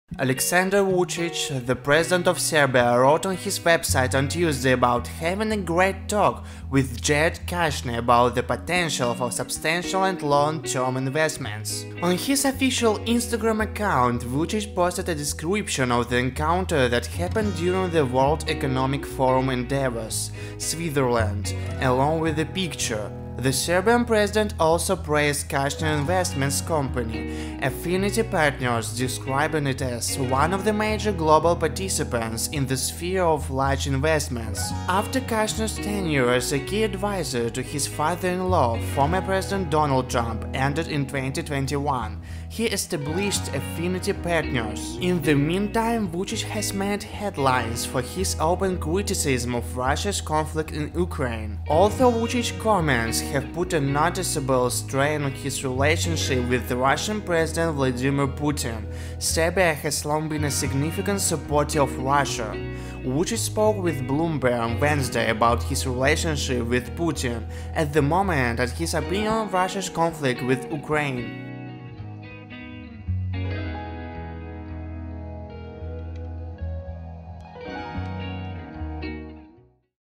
Russia Ally Eyes Business Deal With Jared Kushner
Aleksandar Vucic, the president of Serbia, wrote on his website on Tuesday about having a "great talk" with Jared Kushner about the "potential for substantial and long-term investments."
On his official Instagram account, Vucic posted a description of the encounter that happened during the World Economic Forum in Davos, Switzerland, along with a picture. The Serbian president also praised Kushner's investment company, Affinity Partners, describing it as "one of the major global participants in the sphere of large investments."